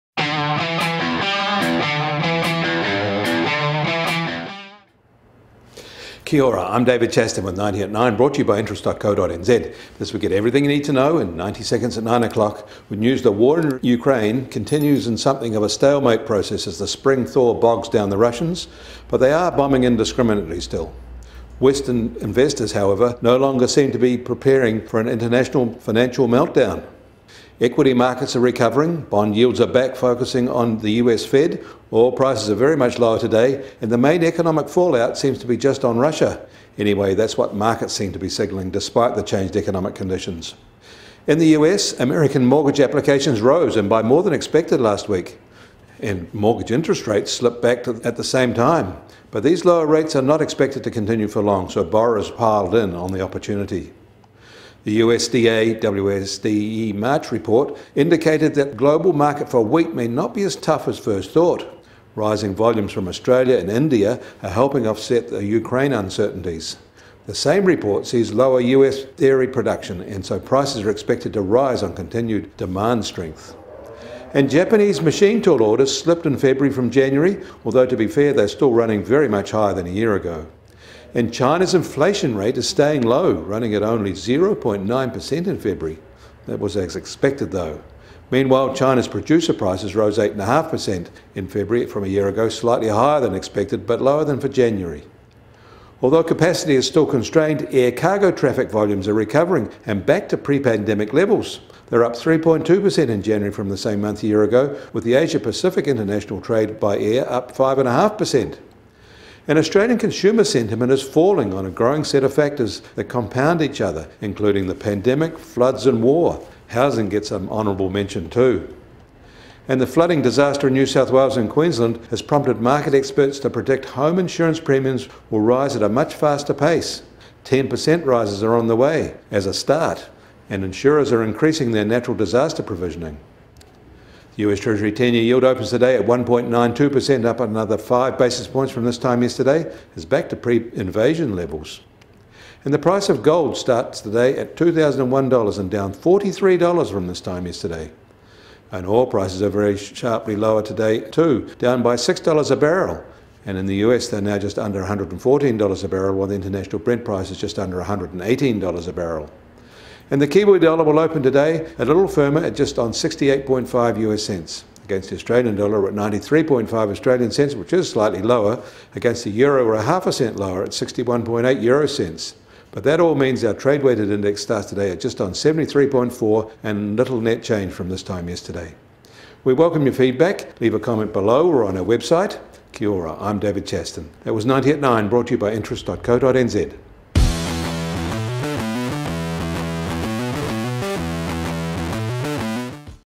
90@9am Moving on from war already
We summarise key economic events overnight with news the war in Ukraine continues in something of a stalemate process as the spring thaw bogs down the Russians. But they are bombing indiscriminately. Still, Western investors no longer seem to be preparing for an international meltdown.